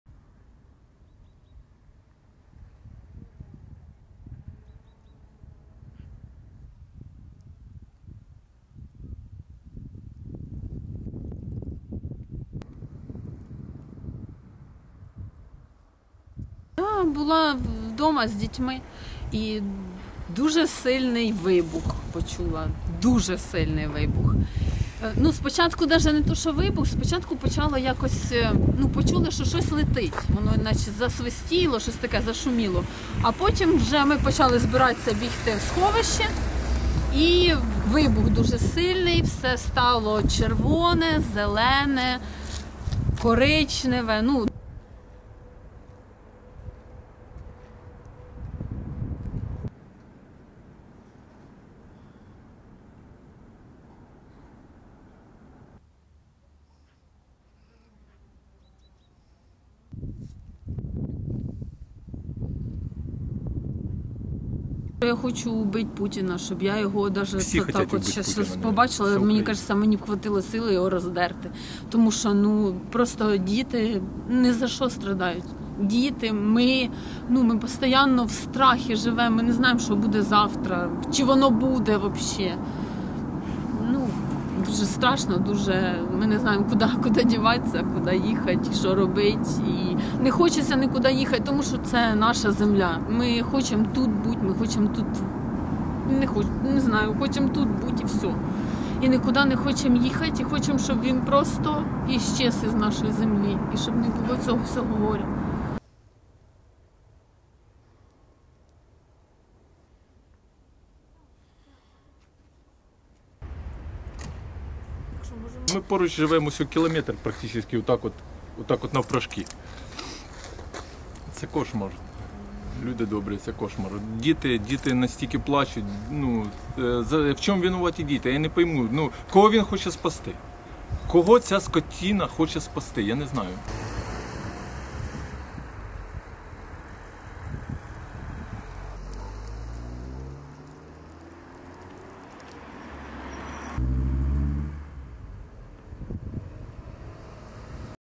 Fuel depot in Kyiv region burning after Russian attack
(25 Mar 2022) Residents living in Kalynovka village near Kyiv have described the moment a fuel depot in the area was hit by Russian fire.
Smoke could be seen emerging from the depot after an explosion late Thursday.
Local residents described it as a missile.
"Firstly I even could hear something flying, there was a whistle. It was very loud. We were on our way to the shelter when heavy explosion happened. And everything became red, green, brown," said Maiya, who lives in the area.
Despite constant shelling, local residents say they still want to stay.
"We don`t know what we should do, where we should hide. We don`t want to leave anywhere, because it's our land," said Maiya.